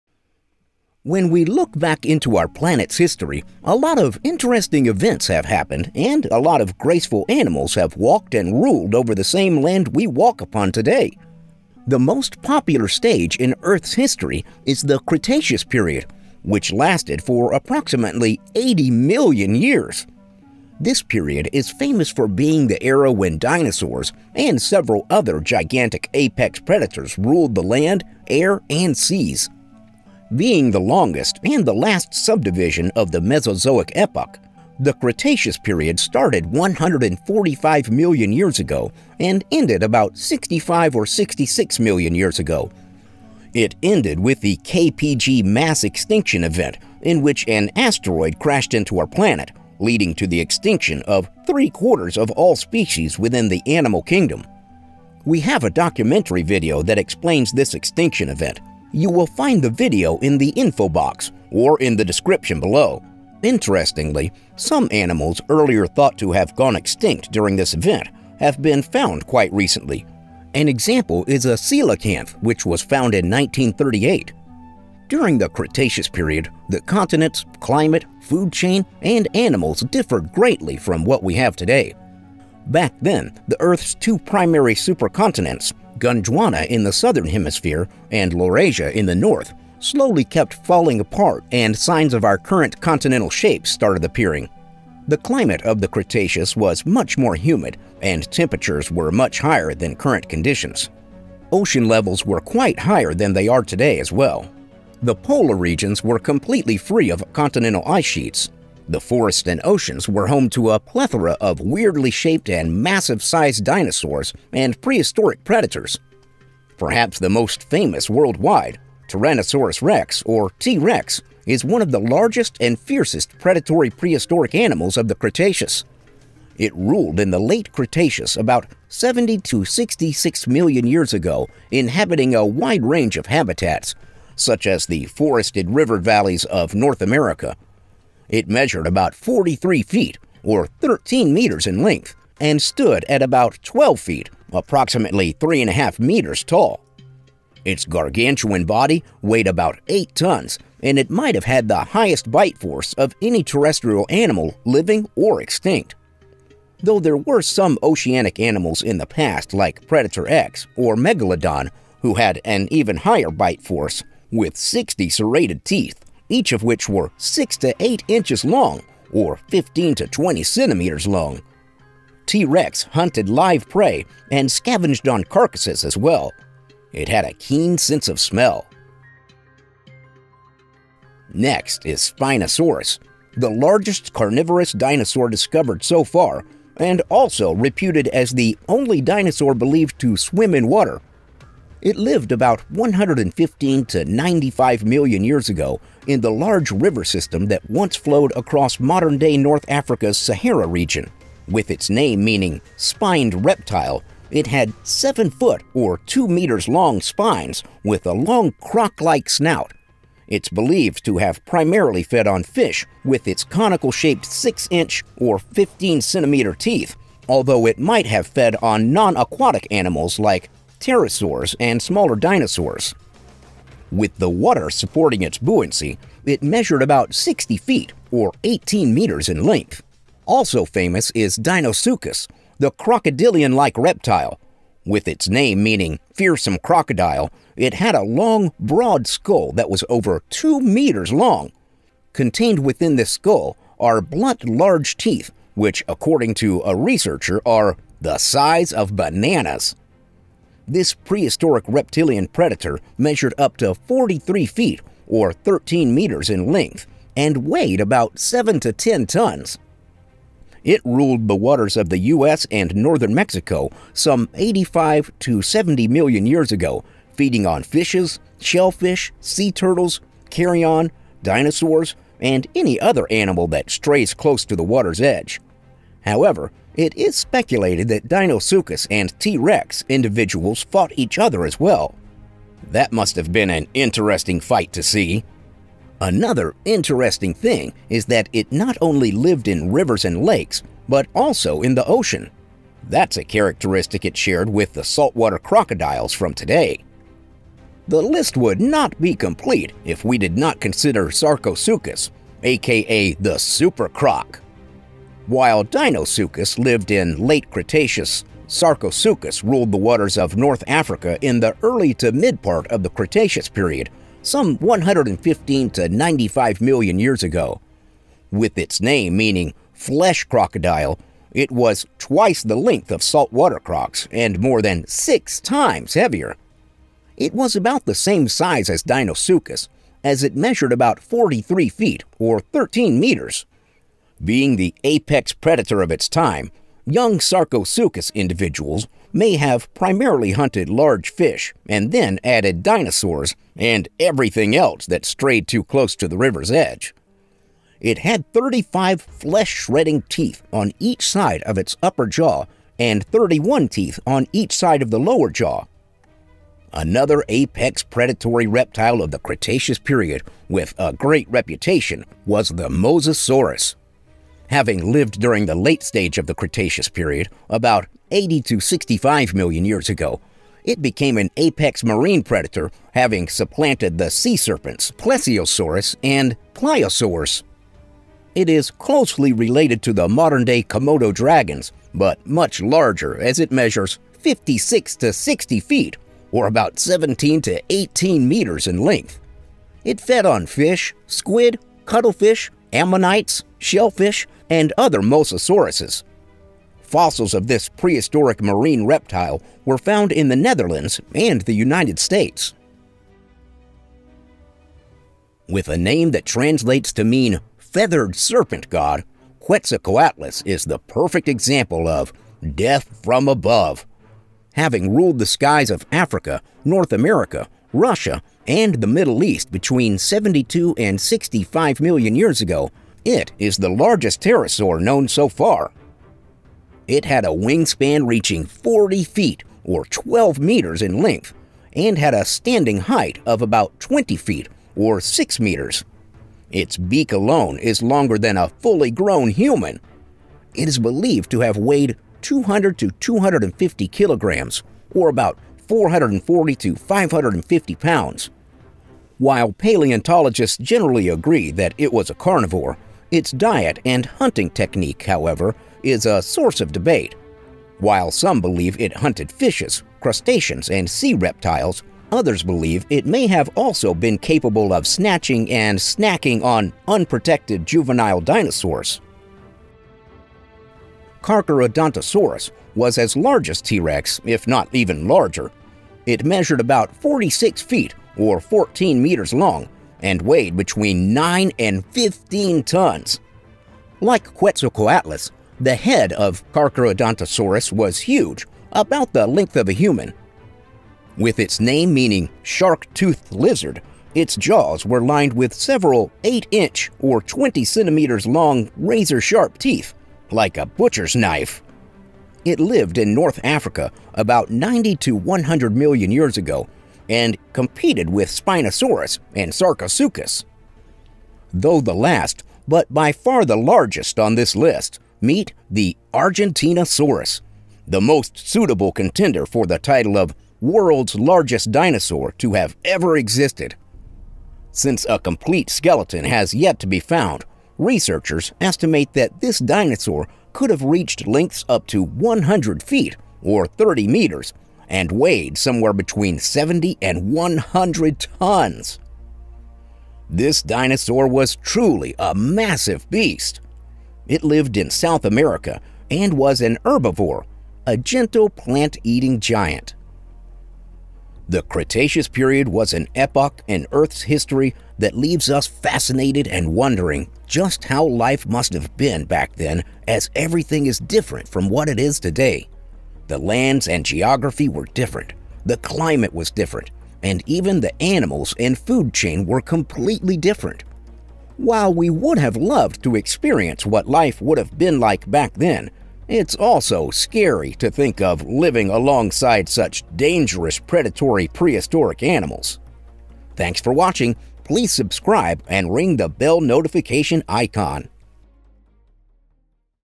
Cretaceous Period - Age Of The Dinosaurs / Documentary (English/HD)
Being the longest and the last subdivision of the Mesozoic epoch, the Cretaceous Period started 145 million years ago and ended about 65 or 66 million years ago. It ended with the K-Pg mass extinction event in which an asteroid crashed into our planet, leading to the extinction of three-quarters of all species within the animal kingdom.

During the Cretaceous Period, the continents, climate, food chain, and animals differed greatly from what we have today. Back then, the earth’s two primary supercontinents – Gondwana in the southern hemisphere and Laurasia in the north slowly kept falling apart and signs of our current continental shapes started appearing.

The climate of the Cretaceous was much more humid and temperatures were much higher than current conditions. Ocean levels were quiet higher than they are today as well. The Polar Regions were completely free of continental ice sheets. The forests and oceans were home to a plethora of weirdly shaped and massive-sized dinosaurs and prehistoric predators.

Perhaps the most famous worldwide, Tyrannosaurus rex or T-rex is one of the largest and fiercest predatory prehistoric animals of the Cretaceous. It ruled the Late Cretaceous about 70 to 66 million years ago inhabiting a wide range of habitats such as the forested river valleys of North America. It measured about 43 feet (or 13 meters in length) and stood at about 12 feet (approximately 3 and a half meters) tall.

Next is Spinosaurus, the largest carnivorous dinosaur discovered so far and also reputed as the only dinosaur believed to swim in water. It lived about 115 to 95 million years ago in the large river system that once flowed across modern-day North Africa’s Sahara region.

Also famous is Deinosuchus, the crocodilian-like reptile. With its name meaning “fearsome crocodile”, it had a long, broad skull that was over 2 meters long! Contained within this skull were blunt large teeth which according to a researcher are “the size of bananas”! This prehistoric reptilian predator measured up to 43 feet (or 13 meters ) in length and weighed about 7-10 tons.

This list would be incomplete if we do not consider Sarcosuchus a.k.a. the “SuperCroc”! While Deinosuchus lived in Late Cretaceous, Sarcosuchus ruled the waters of North Africa in the early to mid-part of the Cretaceous period some 115 to 95 million years ago. With its name meaning “flesh crocodile, it was twice the length of Saltwater crocs and more than 6 times heavier! It was about the same size as Deinosuchus as it measured about 43 feet (or 13 meters).

Another apex predatory reptile of the Cretaceous period with a great reputation was the Mosasaurus. Having lived during the late stage of the Cretaceous about 80 to 65 million years ago, it became an apex marine predator having supplanted the sea serpents – Plesiosaurs and Pliosaurs.

With a name that translates to mean “feathered serpent god”, Quetzalcoatlus is the perfect example of ‘death from above’! Having ruled the skies of Africa, North America, Russia, and the Middle East between 72 and 65 million years ago, it is the largest Pterosaur known so far!

Distantly related to T-rex, Carcharodontosaurus was as large as T-rex if not even larger. It measured about 46 feet (or 14 meters) long and weighed between 9 and 11 tons! Like Quetzalcoatlus, the head of Carcharodontosaurus was huge, about the length of a human.

Though the last, but by far the largest on this list, meet the Argentinosaurus – the most suitable contender for the title of ‘world’s largest dinosaur’ to have ever existed! Since a complete skeleton has yet to be found, researchers estimate that this dinosaur could have reached lengths of up to 100 feet (or 30 meters) and weighed somewhere between 70 and 100 tons. This dinosaur was truly a massive beast! It lived in South America and was an herbivore – a gentle plant-eating giant.

The Cretaceous Period was an epoch in earth’s history that leaves us fascinated and wondering just how life must have been back then as everything is different from what it is today. The lands and geography were different, the climate was different and even the animals and food chain were completely different. While we would have loved to experience what life would have been like back then, it’s also scary to think of living alongside such dangerous predatory prehistoric animals.

Narrator: Larry G. Jones